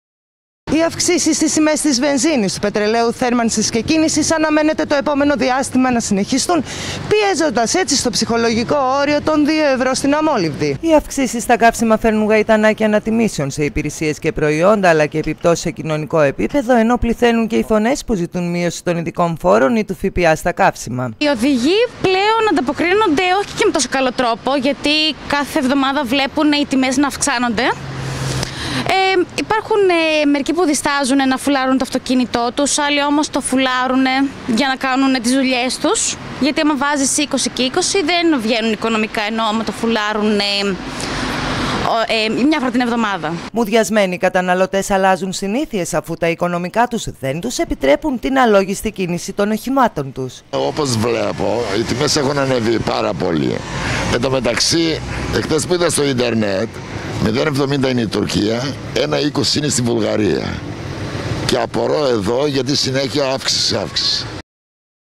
Αυξήσεις στα καύσιμα - Στα ύψη οι τιμές της βενζίνης
Σταθερά ανοδική πορεία ακολουθούν οι τιμές των καυσίμων ,παρασύροντας τις τιμές  όλων των καταναλωτικών αγαθών με τους πολίτες να ανησυχούν για αυτό που θα ακολουθήσει .